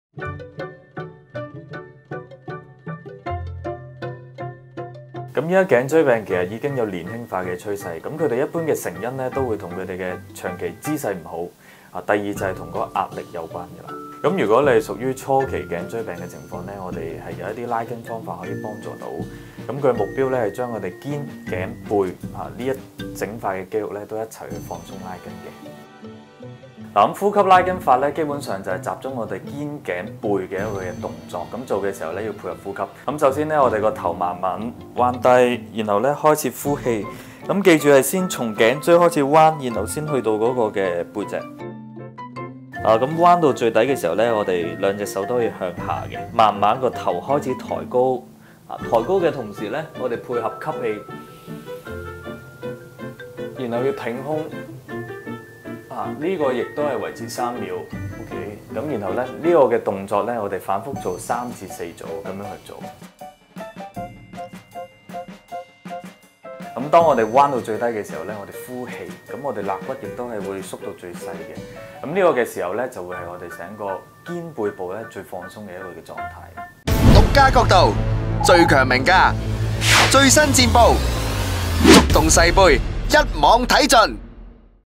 肩頸背痛無有怕 中醫教呼吸拉筋法幫到手
【養生食經】
頸椎病這個名字大家一定聽過，卻好像與自己毫不相干。其實錯了！頸椎病比我們想像的來得更近。以往看頸椎病的年齡層一般都是40歲以上的人士，現在可以看到越來越多的年輕一輩，甚至是剛剛上大學的90後都有頸椎問題。究竟頸椎病是甚麼一回事？